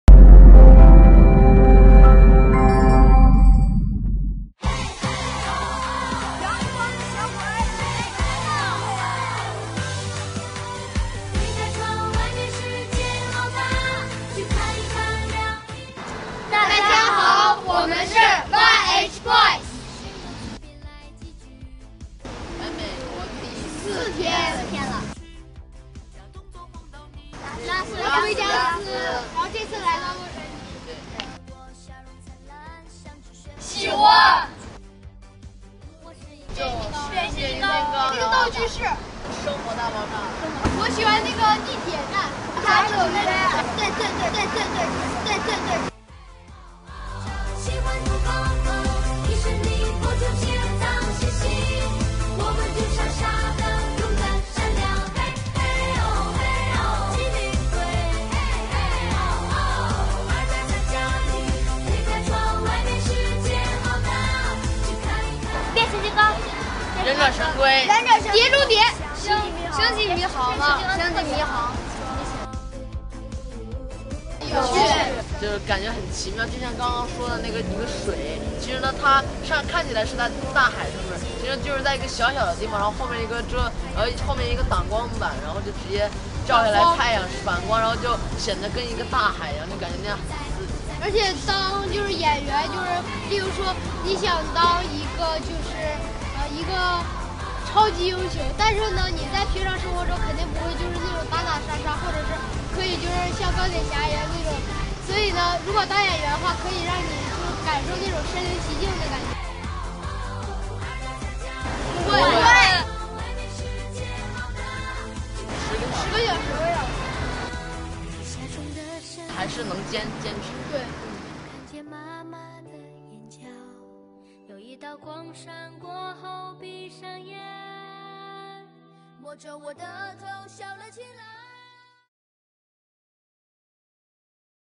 YHBOYS 美国行参观派拉蒙制片厂
cr：北美电影资讯
此前，YHBOYS组合 全体成员赴美参加了在拉斯维加斯举办的2017Billboard 公告牌音乐大奖，并顺道走访了洛杉矶，参观了坐落于好莱坞的派拉蒙制片厂。没想到小小年纪的他们对好莱坞大片竟甚是了解，这次更是成为了《变形金刚》的中国形象大使！刚参加完派拉蒙贵宾游，美少年们各个都好兴奋，看看他们是怎么说的！